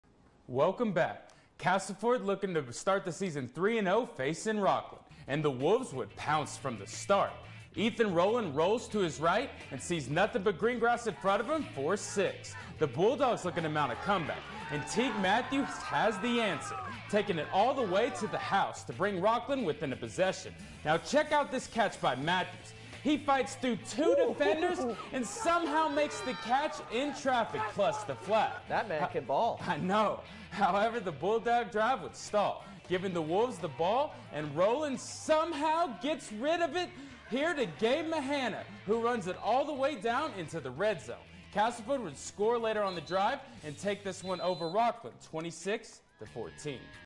Castleford outlasts Rockland 26-14 to advance to 3-0
Castleford outlasts Rockland 26-14 to advance to 3-0 [(918556)]